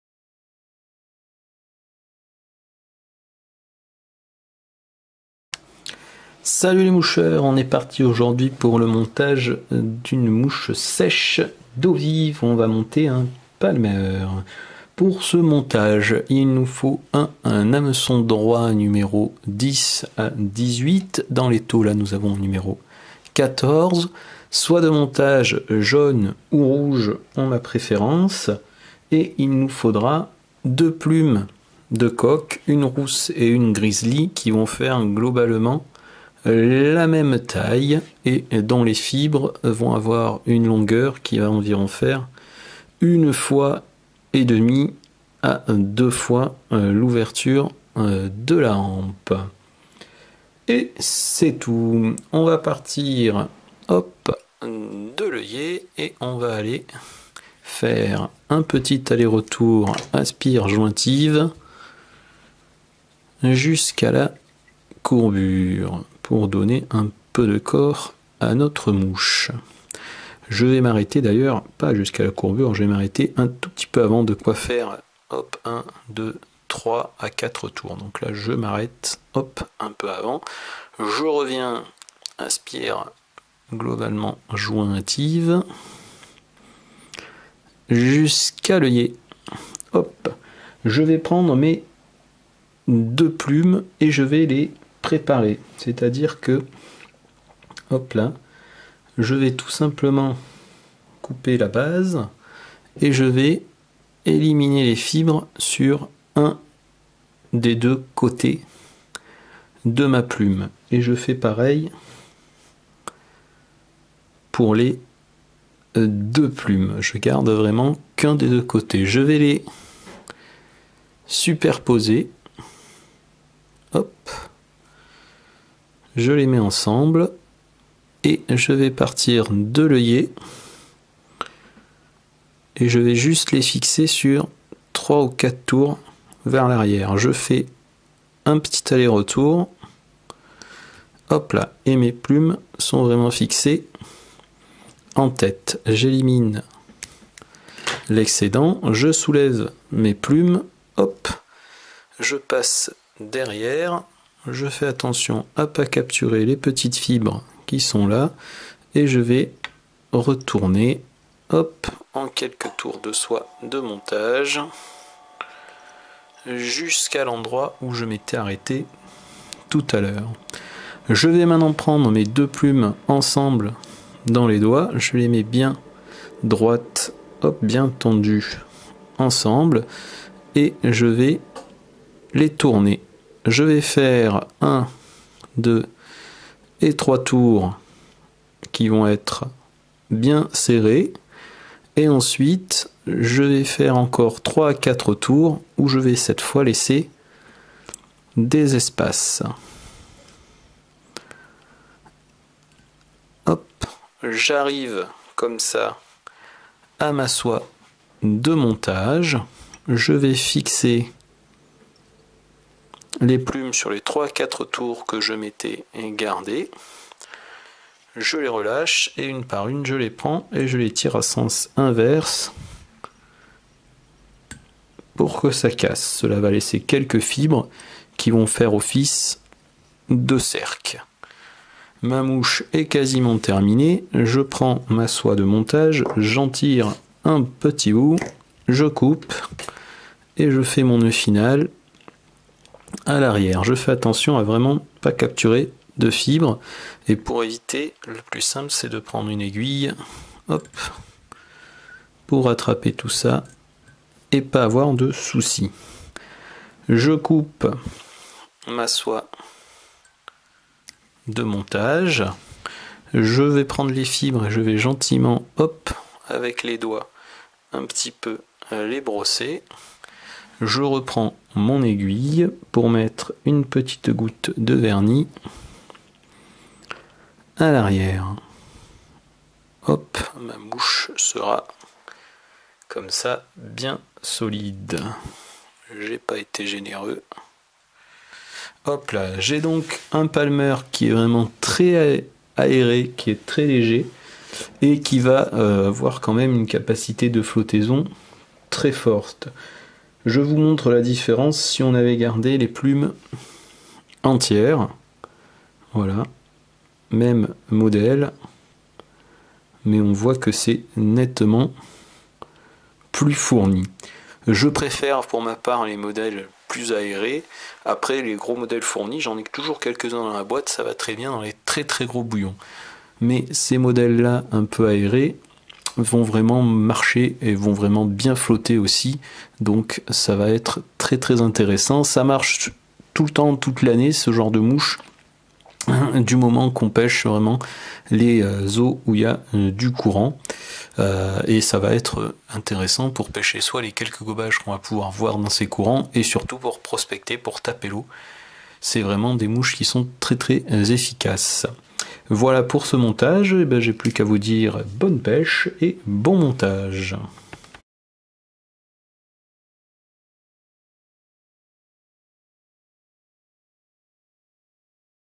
Montage Mouche : Palmer bicolore
Montage d'une mouche de genre palmer.
Mouche légère et très prenante destinée aux eaux rapides.
Le matériel + le montage + l'utilisation tout est dans la vidéo !
Bon montage et bonne pêche